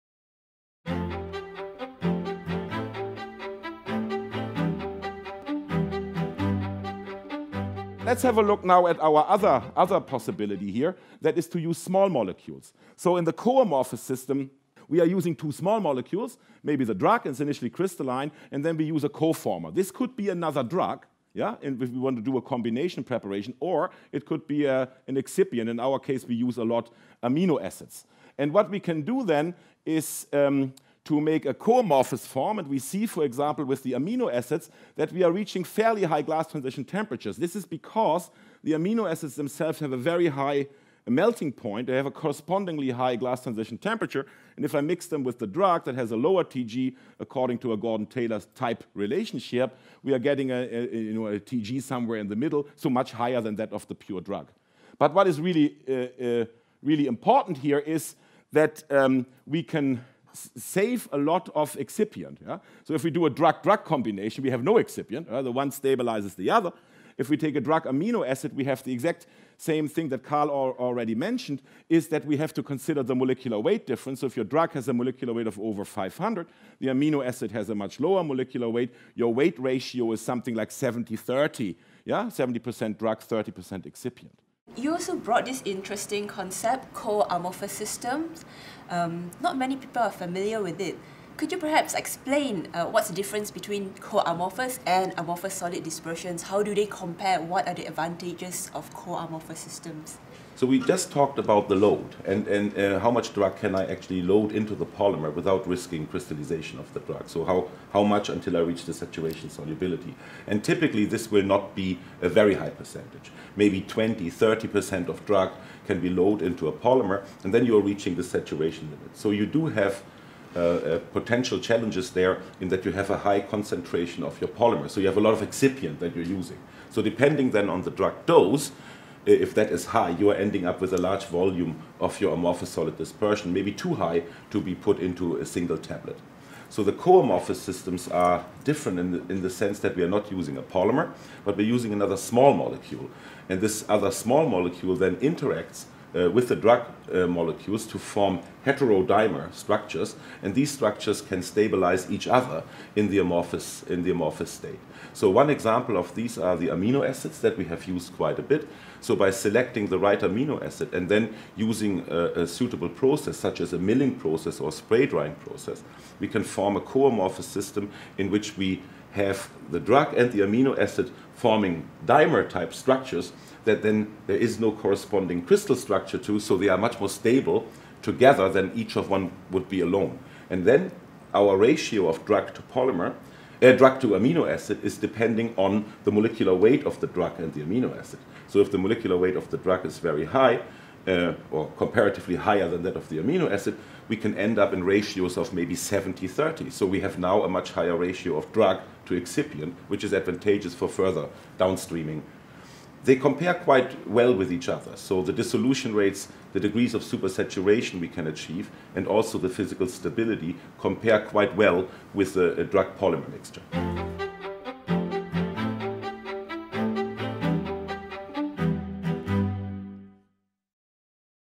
Amorphous versus Co-Amorphous Systems
Not many people are familiar with Co-Amorphous Systems. What’s the difference to Amorphous Systems? And what are the advantages? Dr. Thomas Rades from University of Copenhagen gives an overview of the benefits in using small molecules instead of another polymer.